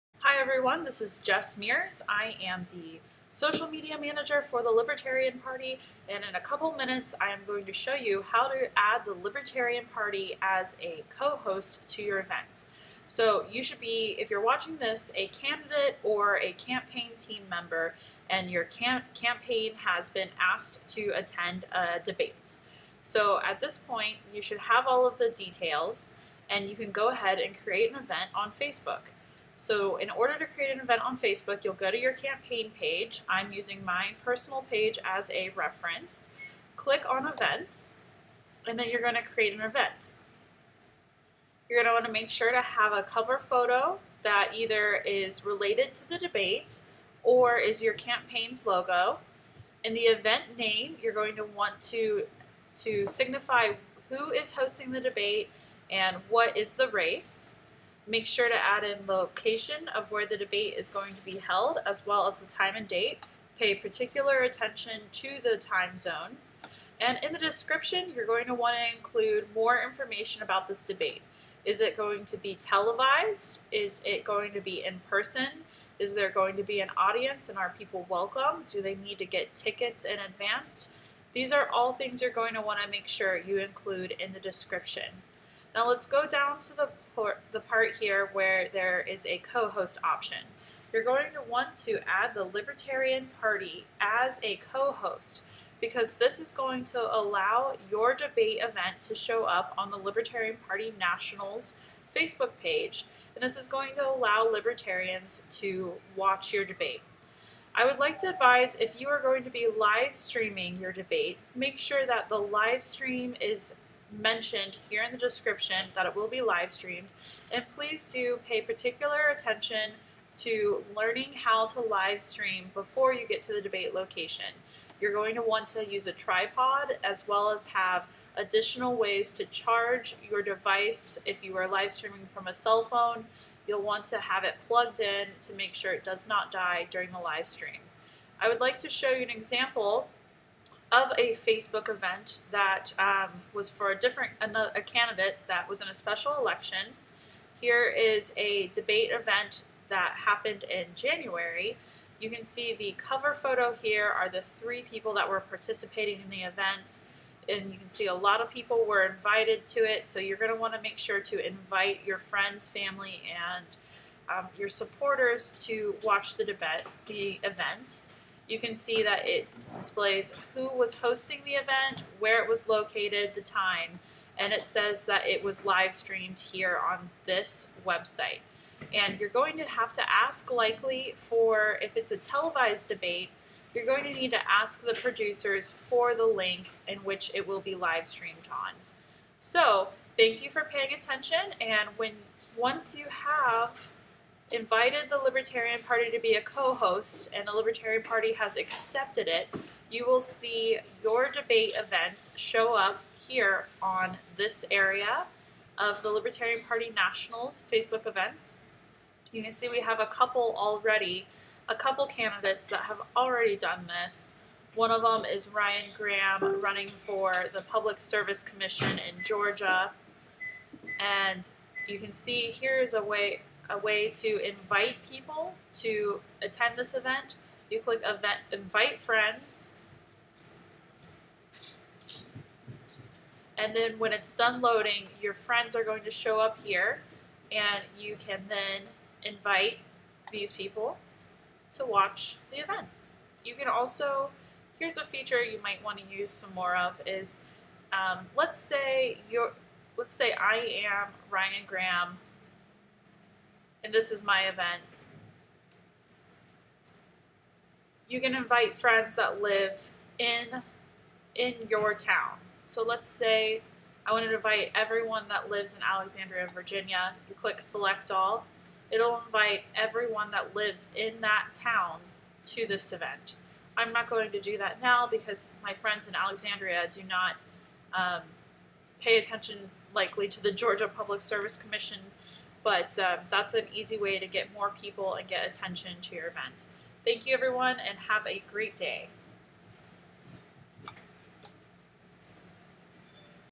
DEBATES 2018: How to add the Libertarian Party as a co host to your FB event
Candidates and campaigns: this is a tutorial on how to add the Libertarian Party's (national) Facebook account as a co-host to your debate Facebook event.

Let's celebrate and cheer on our Libertarian candidates that are included in debates.